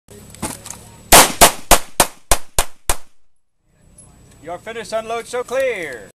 Holding the dog food bag
One of the stages at Universal Shooting Academy's Defensive Pistol League. Great fun and skill building all at the same time.